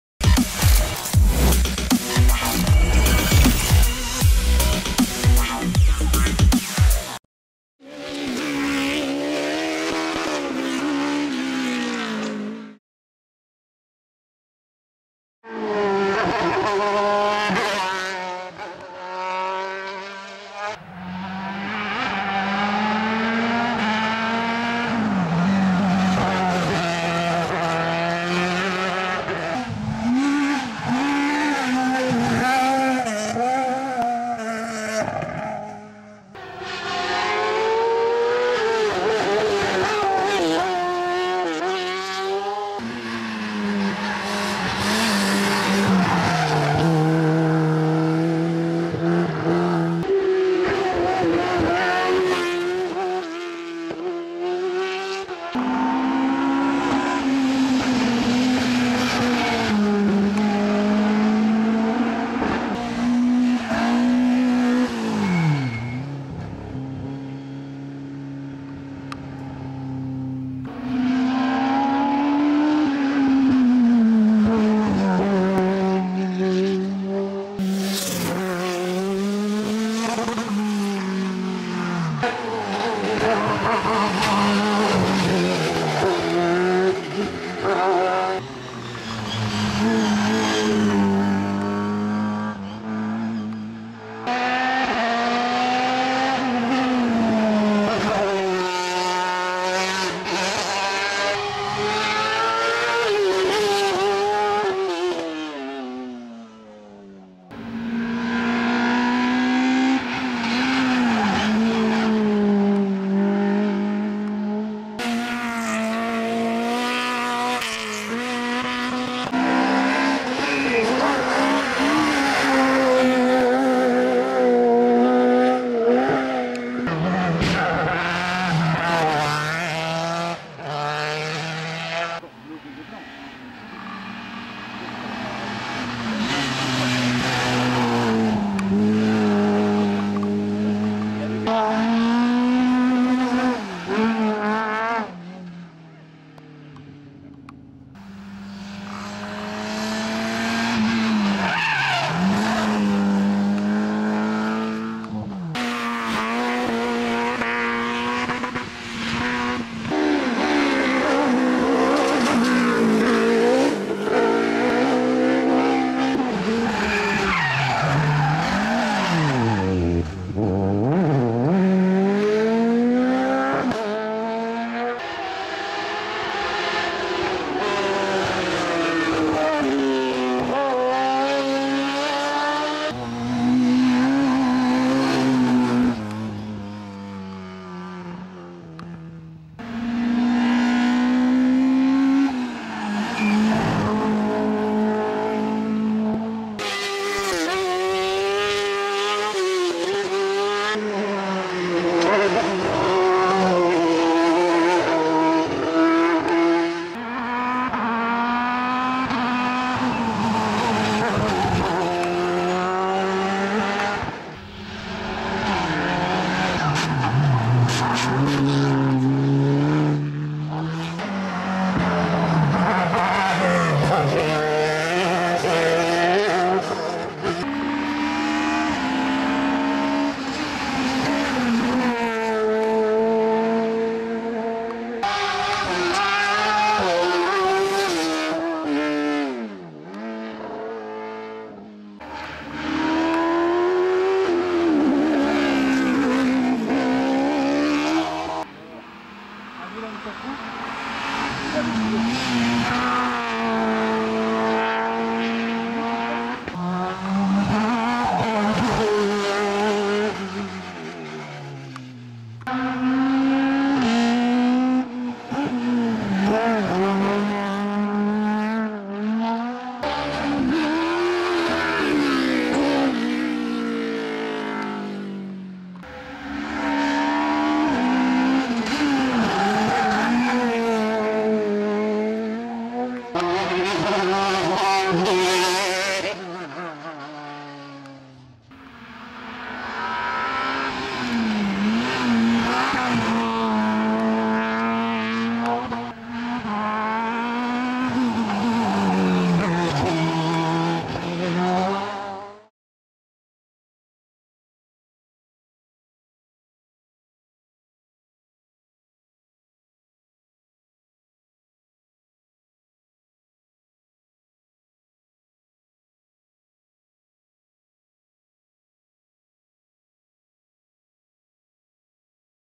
COURSE DE COTE DES MIMOSAS 2020 - SHOW
La 6ème édition de lacourse de cote des mimosas s'est déroulé sous un soleil radieux.
Malgré le faible nombre d'engagés l'attaque fut au rendez-vous.